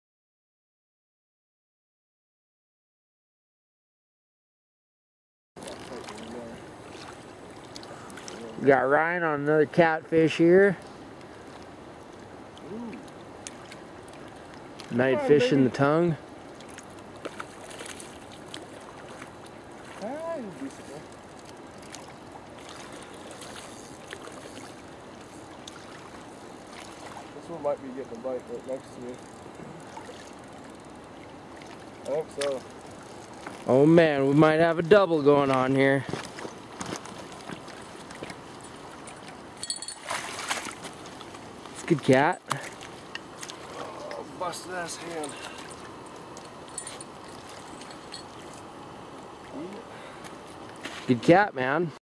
Night Fishing Catfish
Fishing the Tongue River at Night for River Monsters